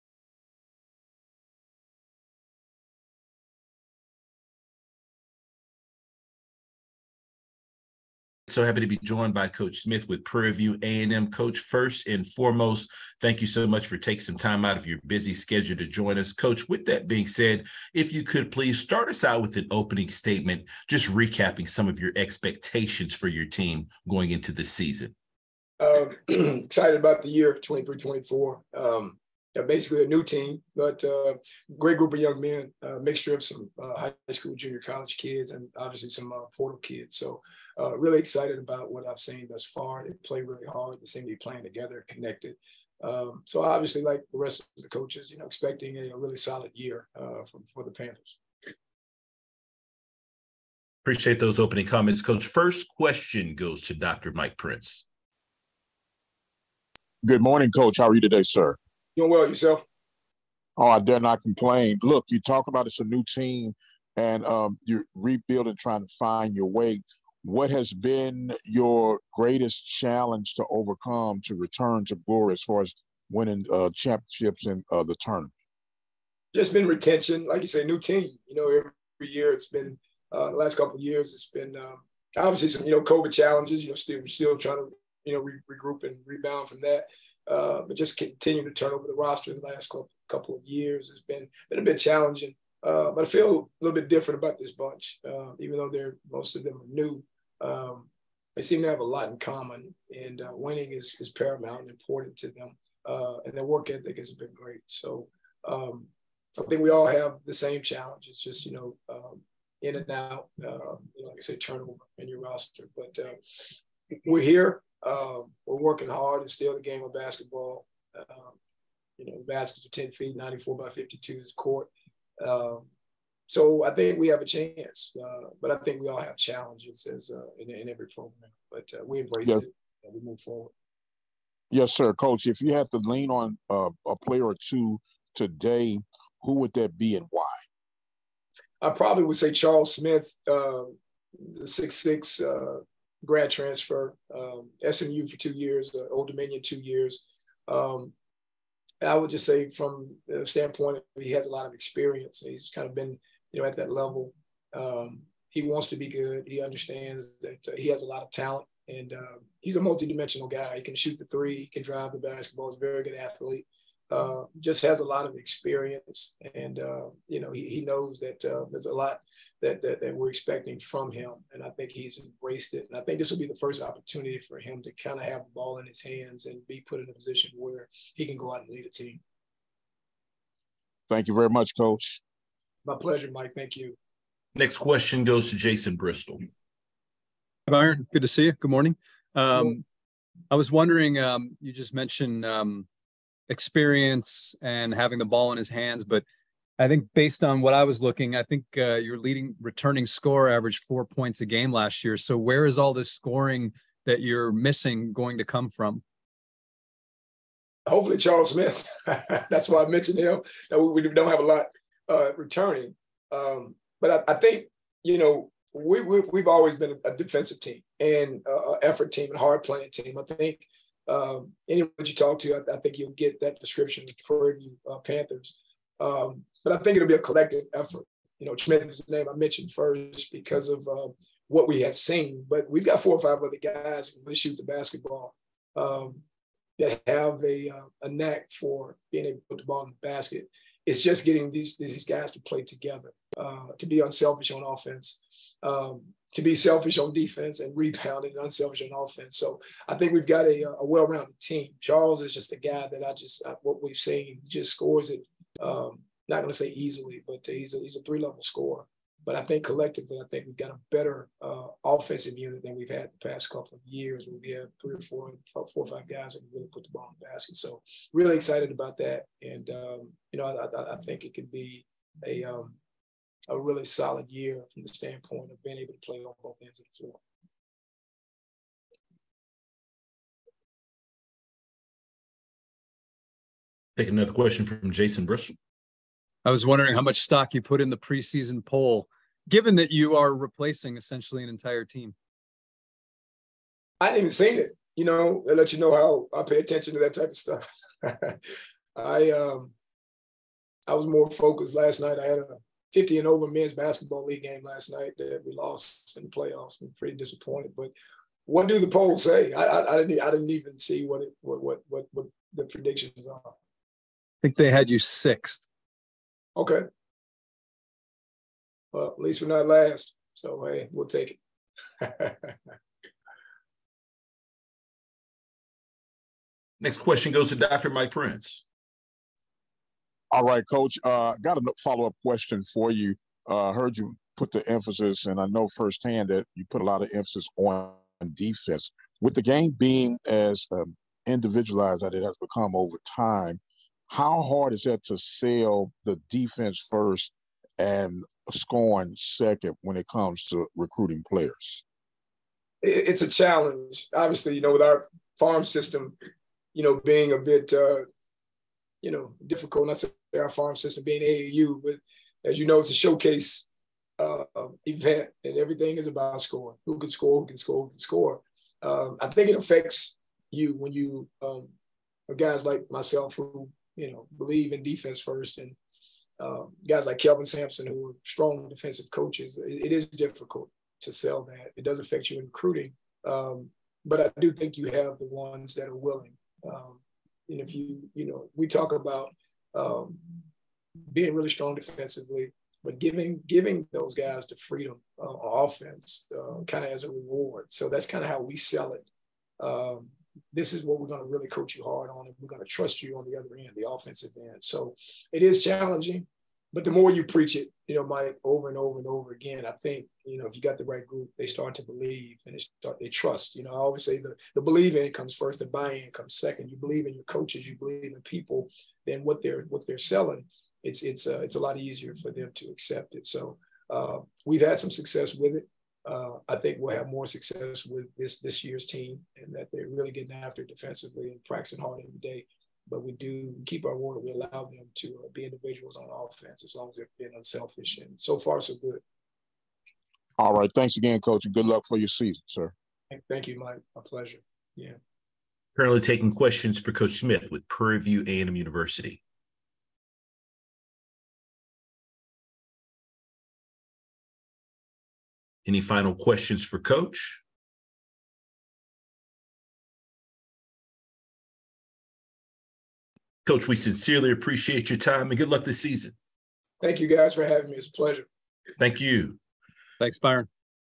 Coach Byron Smith SWAC Media Day
Prairie View A&M University Men's Basketball Coach Byron Smith at the 2023 SWAC Media Day